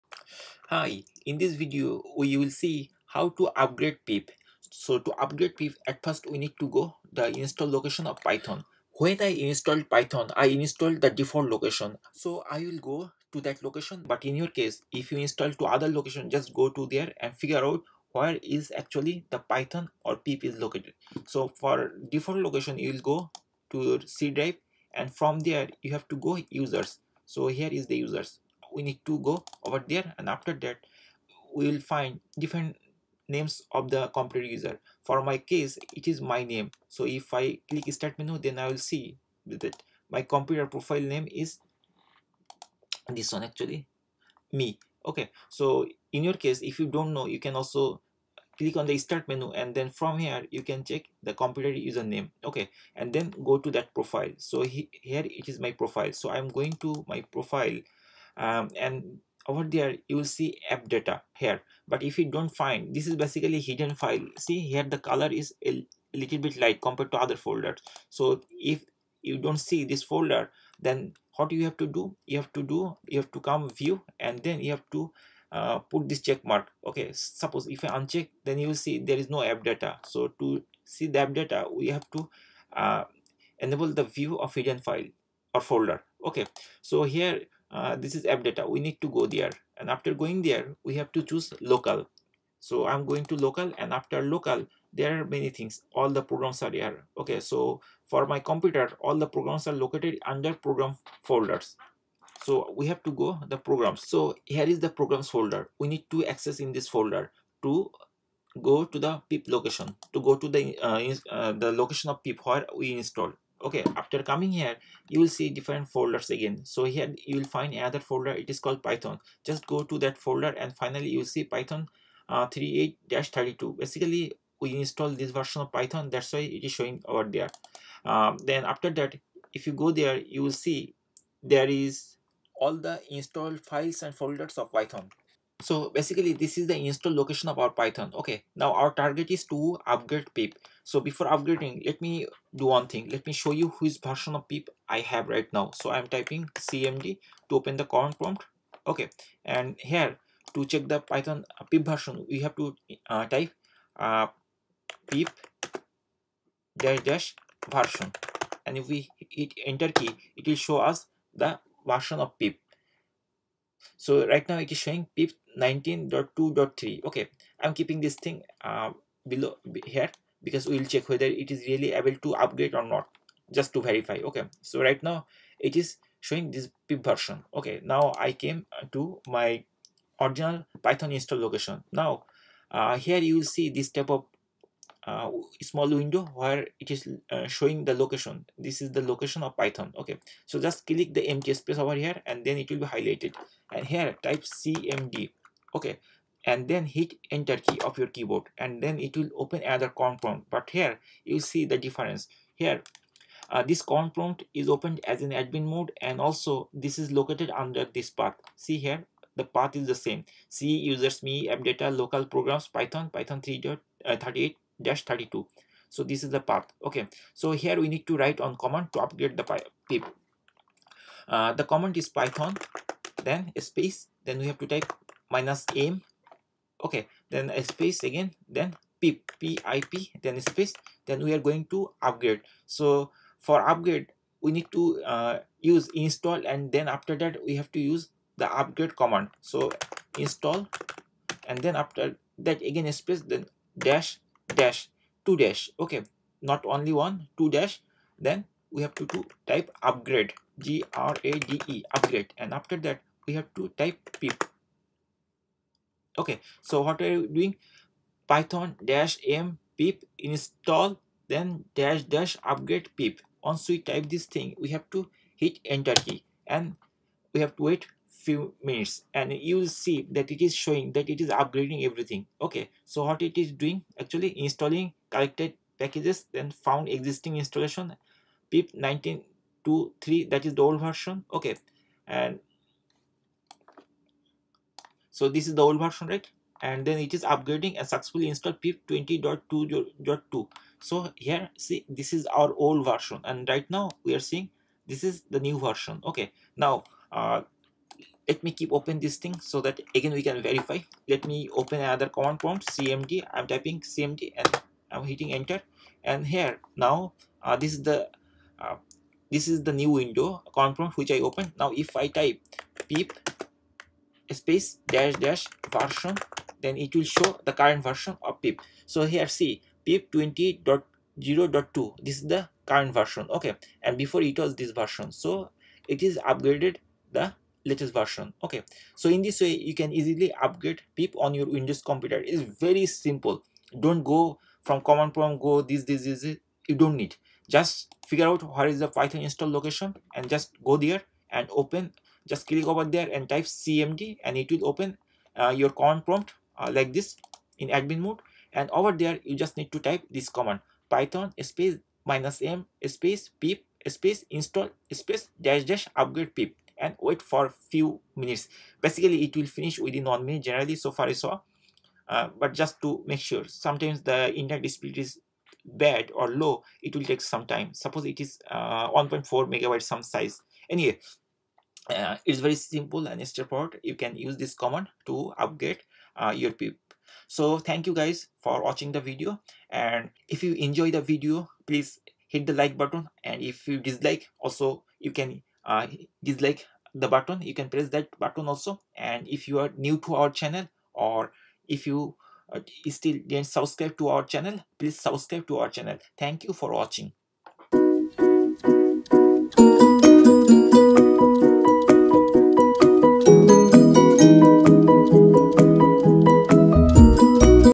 Quick way to Upgrade pip on your windows computer using CMD from Python Install location (--upgrade)
This video explains how to  Quick way to Upgrade pip on your windows computer using CMD from Python Install location (--upgrade)